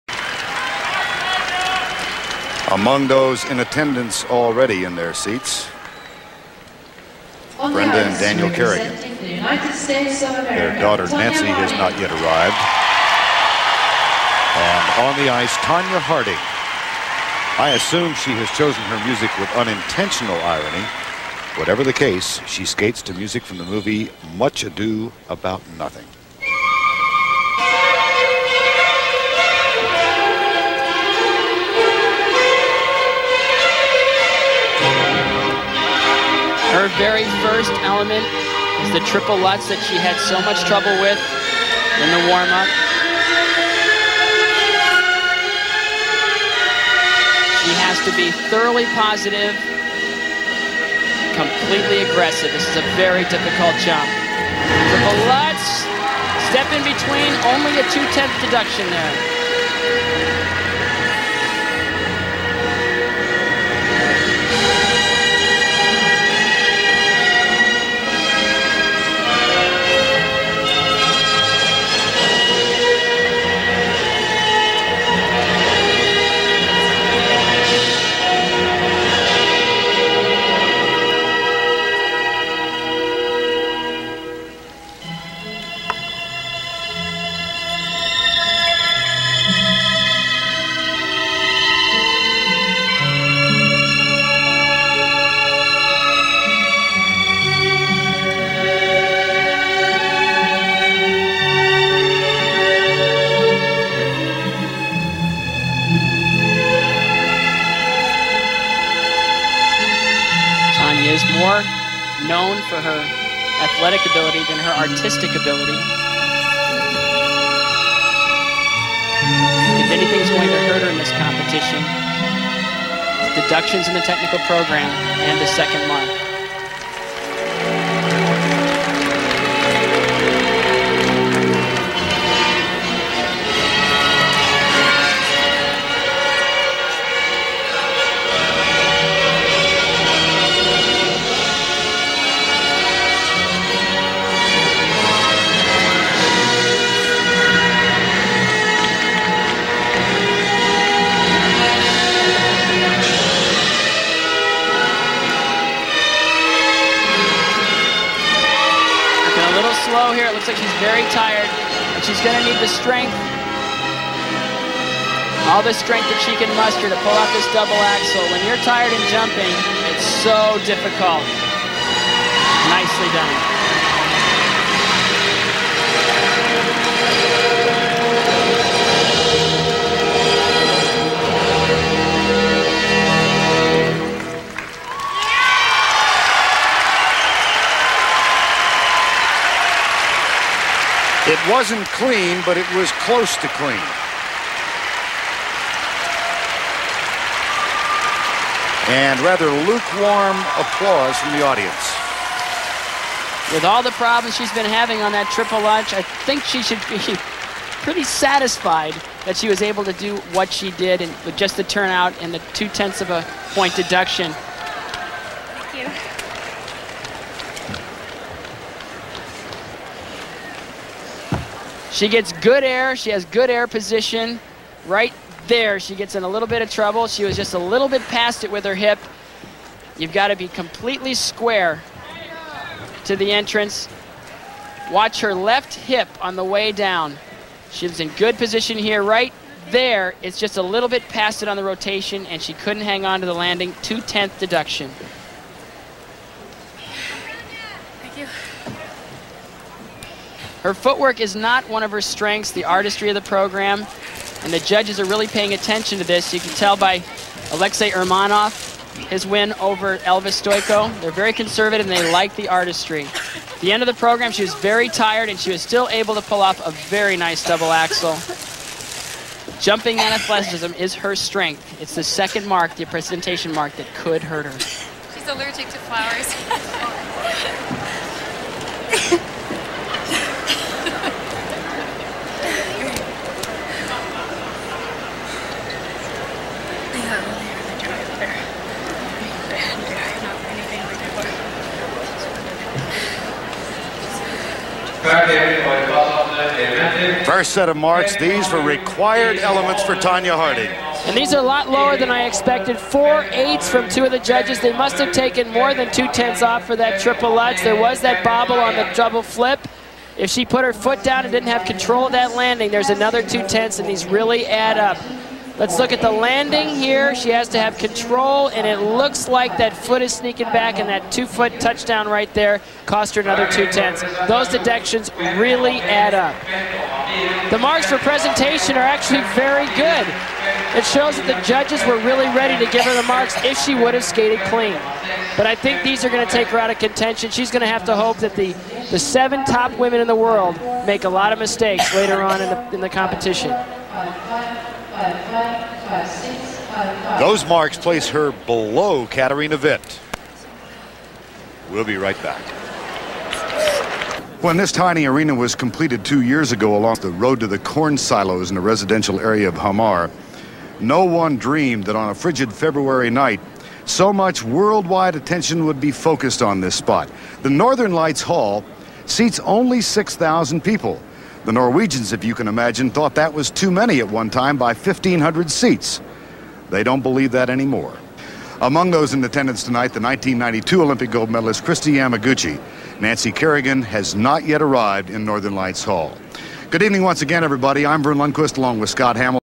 Tonya Harding (USA) - 1994 Lillehammer, Figure Skating, Ladies' Technical Program
Hamar, NORWAY - 1994 Winter Olympics, Figure Skating, Ladies' Technical Program - Tonya Harding of the United States made two errors in this skate, a step out of the triple Lutz in the combination jump and near miss on the landing of the double flip.  The 2 errors would add up to about 0.4 deduction.  And since all the top skaters would skate cleanly, she would end up placing 10th here.